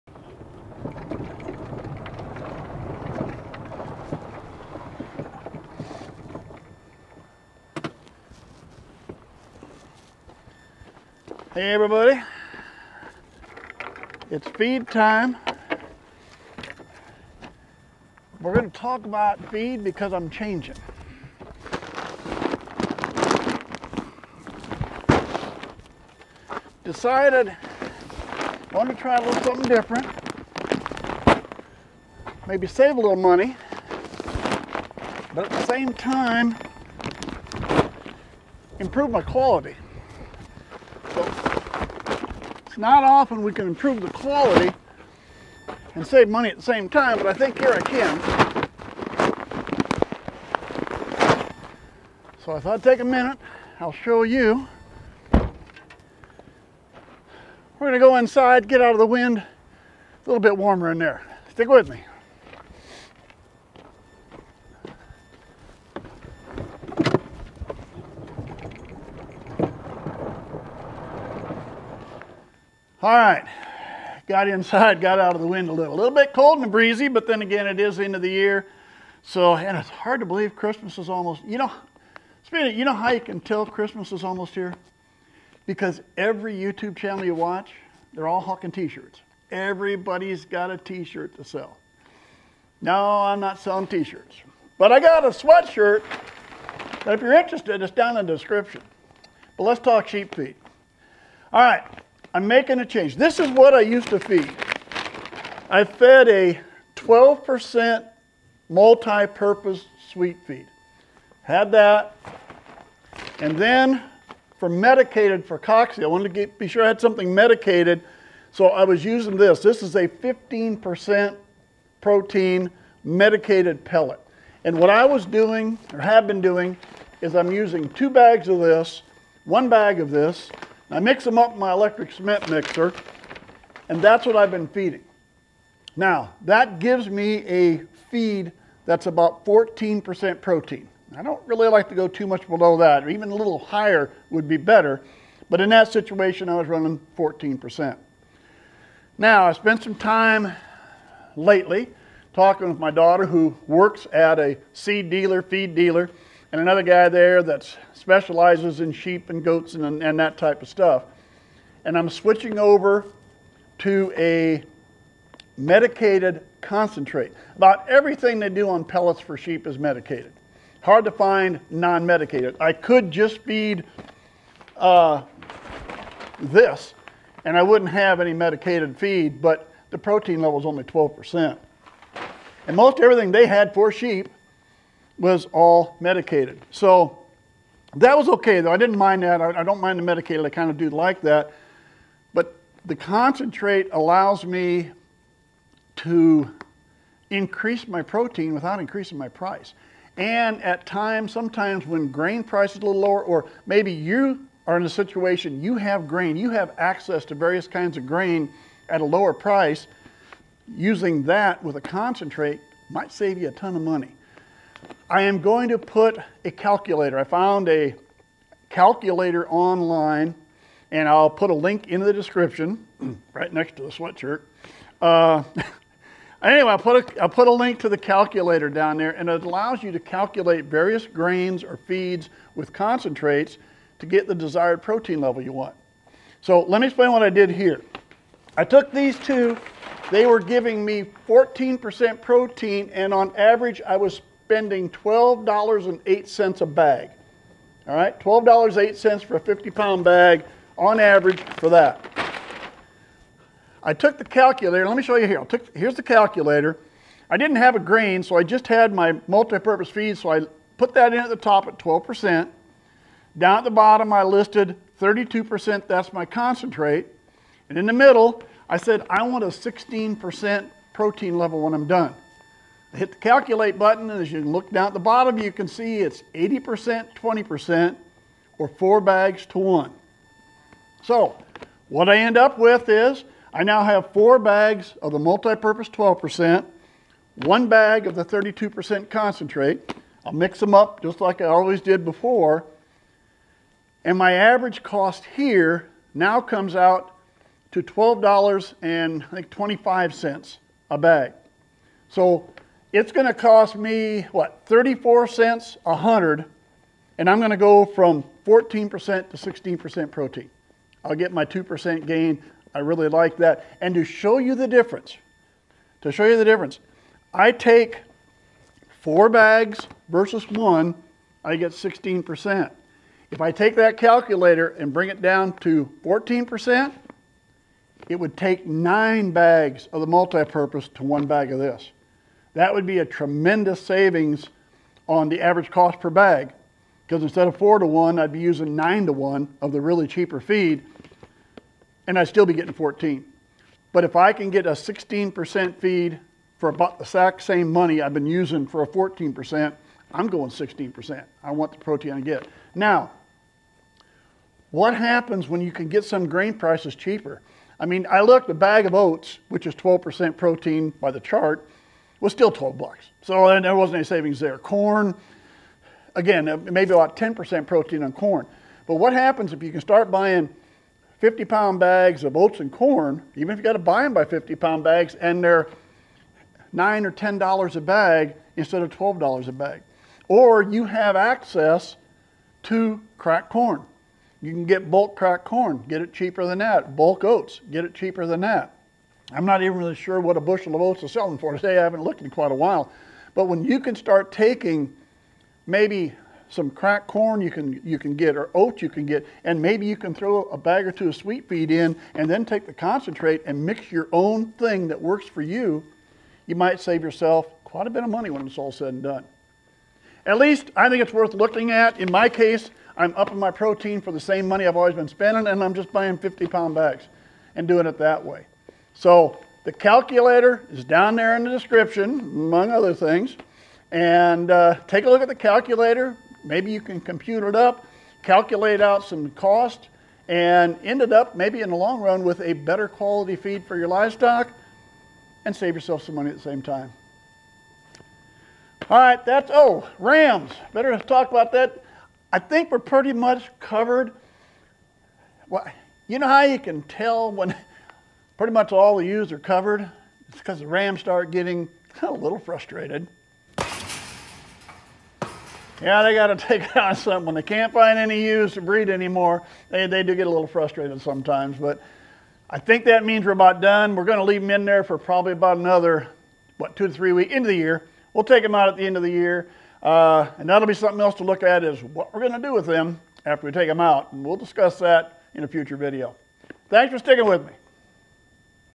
FCTS 42 Feed ration calculator for Painted Desert Sheep
I'm changing my feed ration to gain some protein while keeping my cost the same.